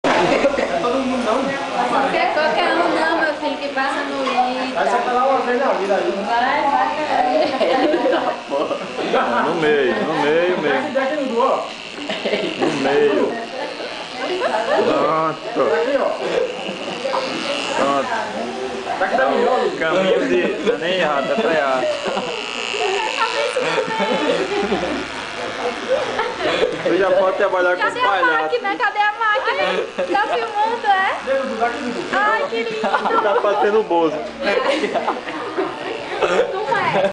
Homero ITA
Raspando a cabeça de Homero!
Aprovaçao no ITA 2008!